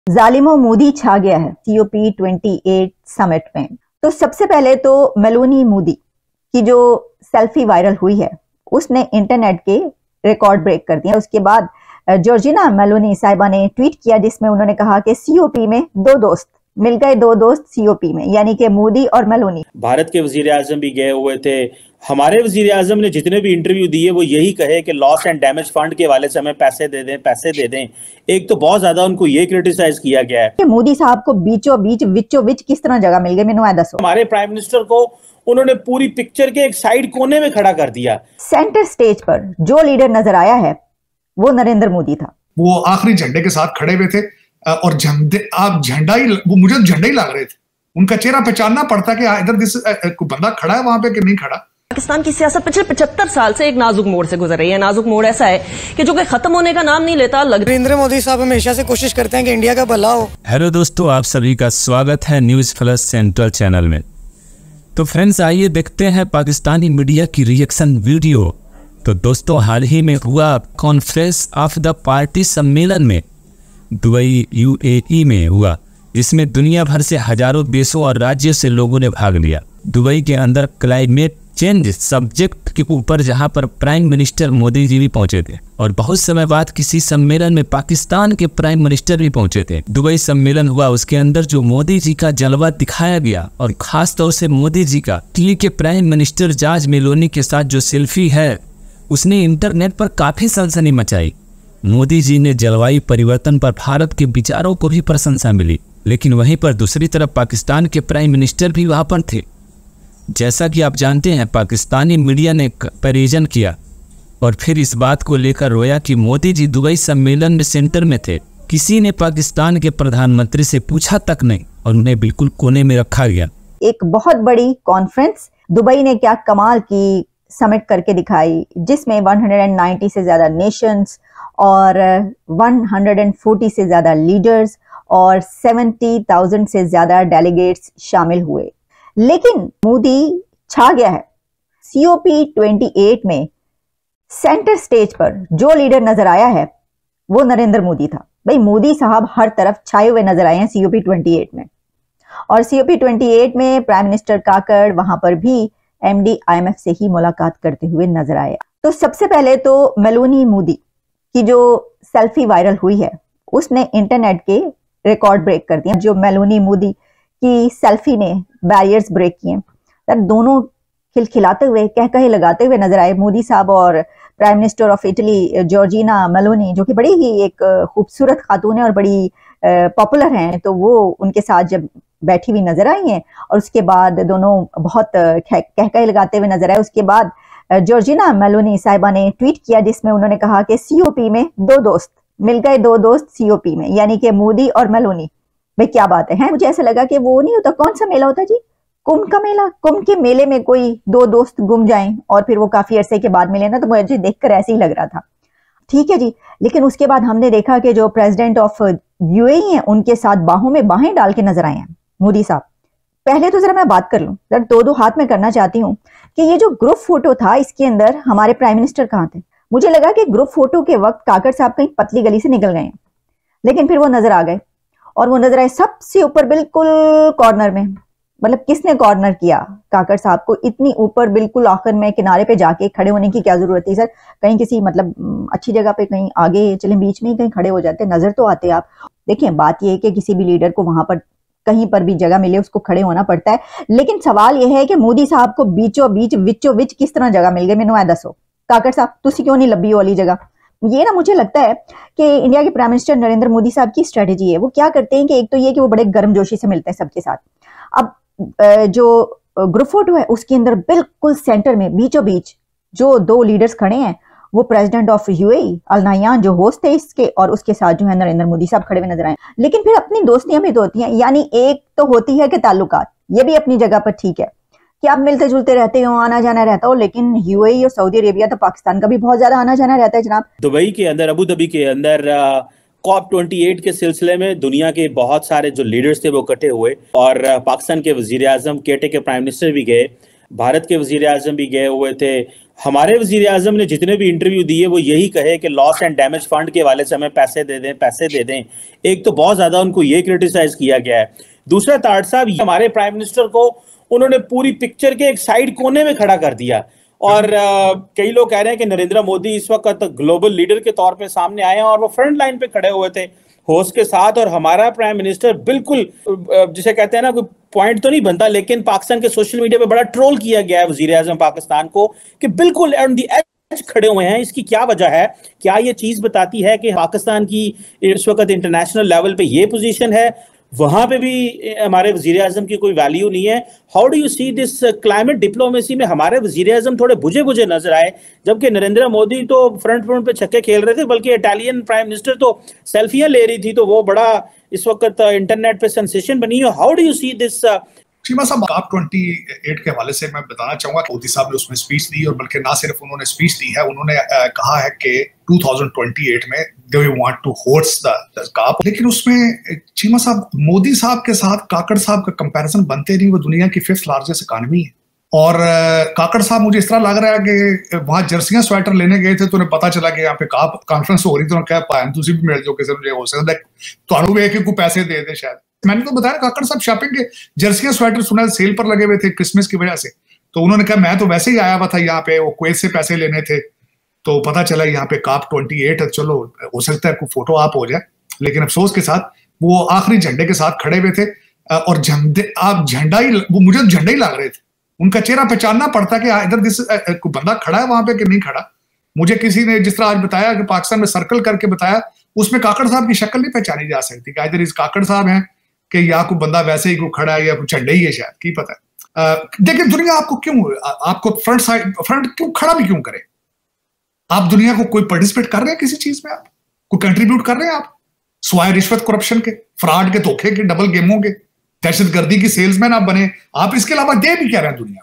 Italy की प्रधानमंत्री Meloni ने Dubai में पीएम Modi के साथ ली सेल्फी | pakistan media crying
About Channel:
NewsFlash Central News Channel Covers The Latest News, Breaking News, Politics, Entertainment News, Business News and Crime News.